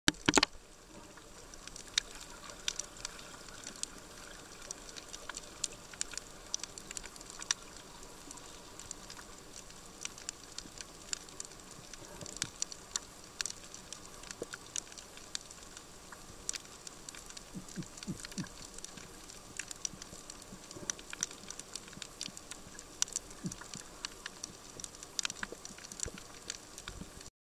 Spinecheek Anemonefish - Raja Ampat
Here is a Spinecheek Anemonefish that we saw in Raja Ampat on a snorkeling live-aboard trip in 2011.